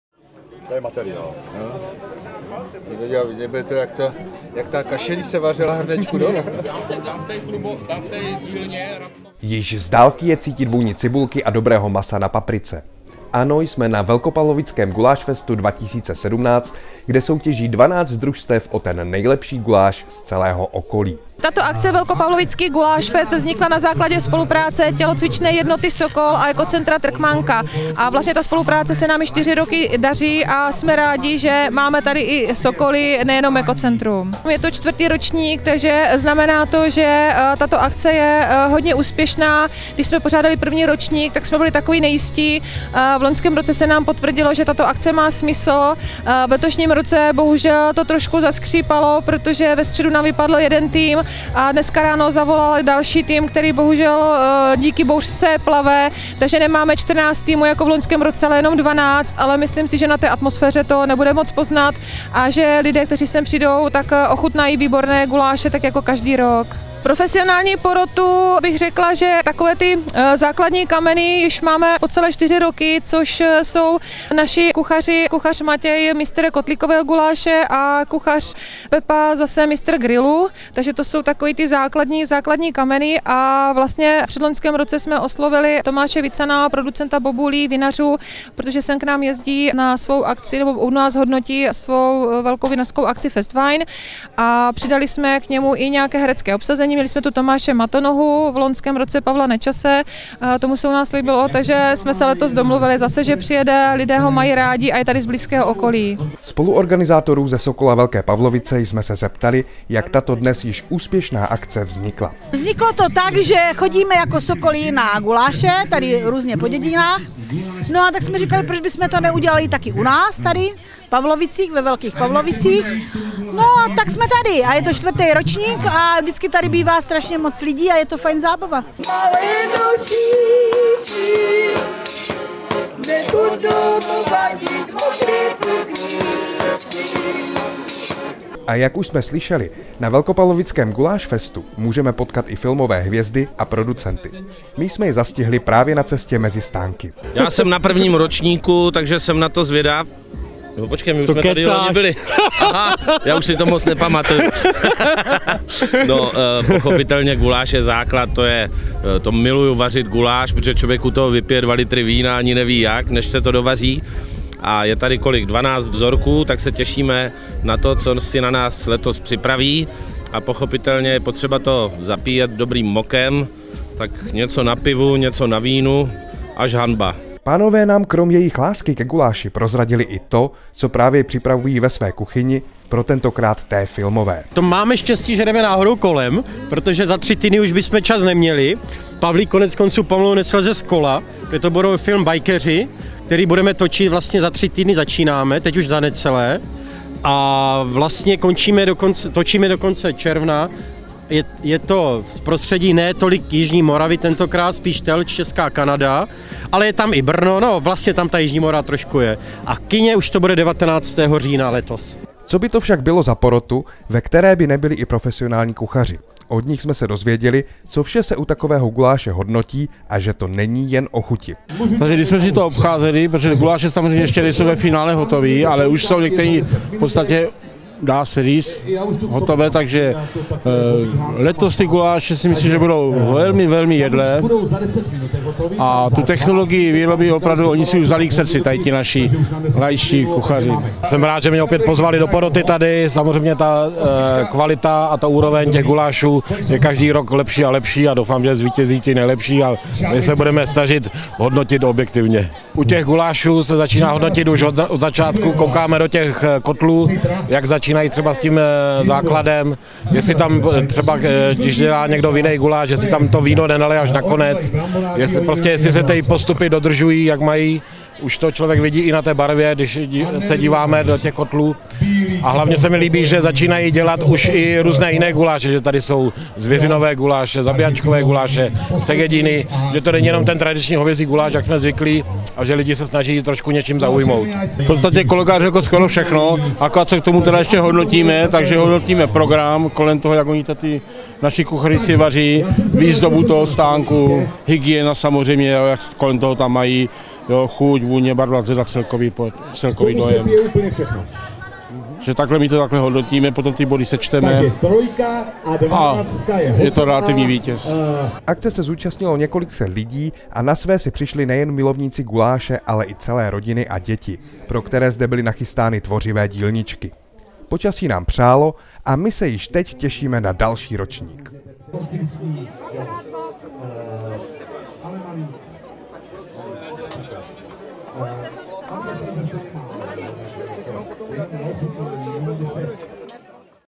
Velké Pavlovice provoněl guláš z dvanácti kotlů
Již z dálky je cítit vůni cibulky a dobrého masa na paprice, ano jsme na Velkopavlovickém Gulášfestu 2017, kde soutěží 12 družstev o ten nejlepší guláš z celého okolí.